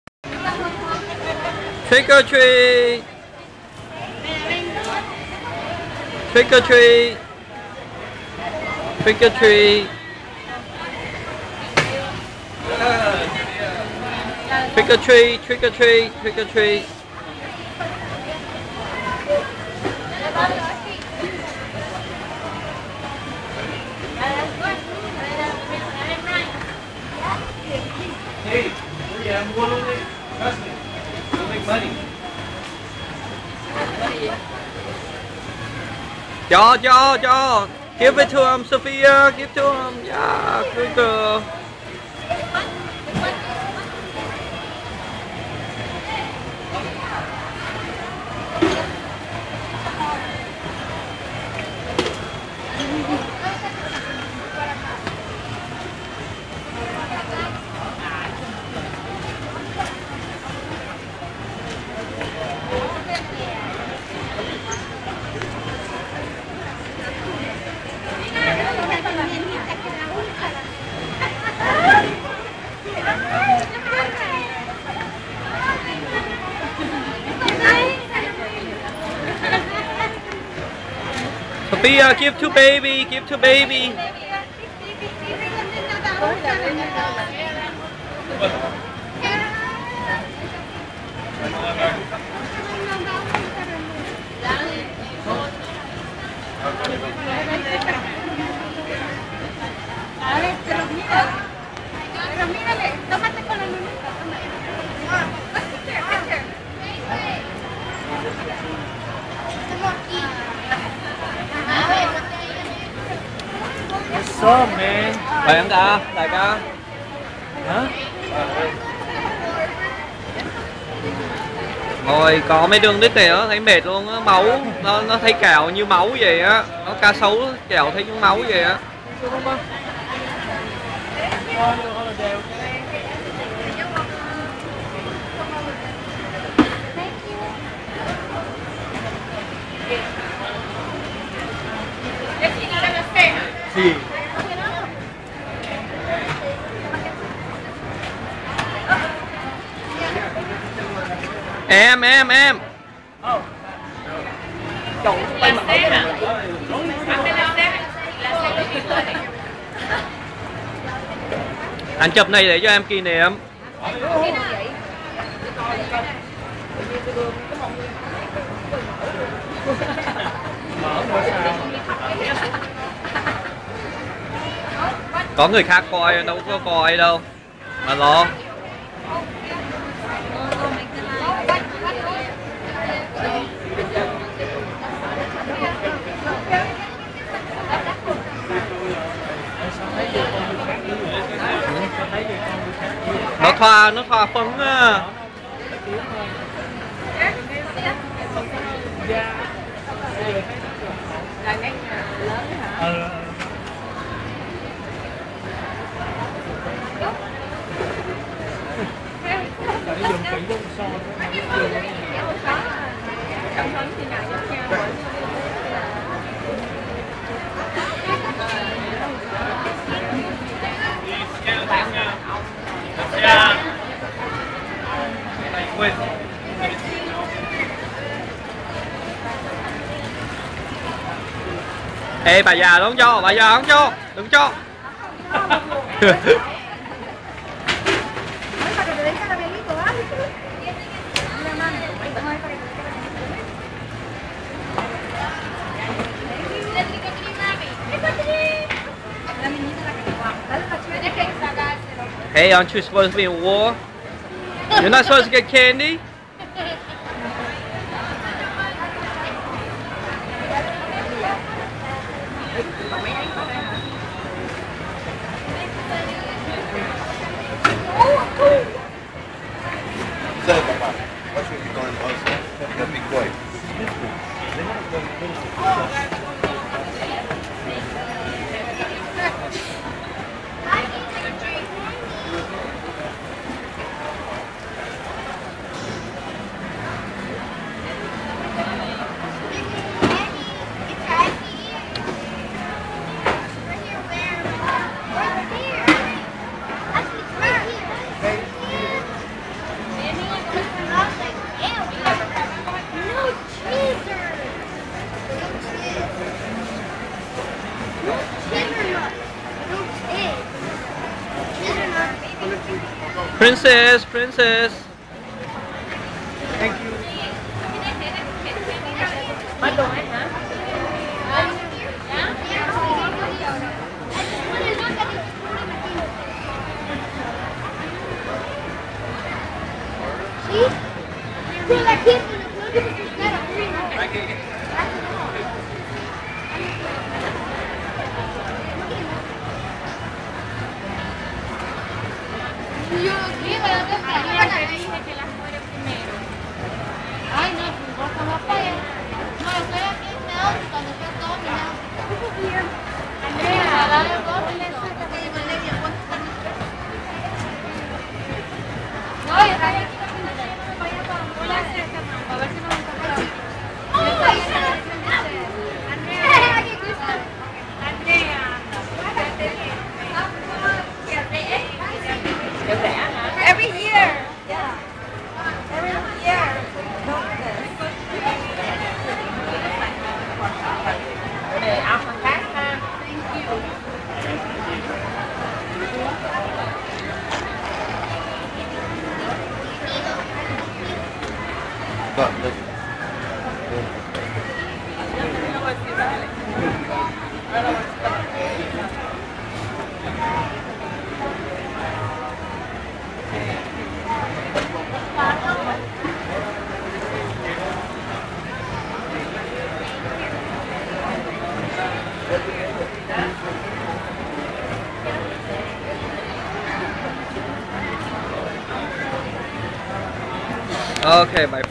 CHAU VAN TRUONG - Sophia Halloween 4
Sophia dress as the ladybug on Halloween.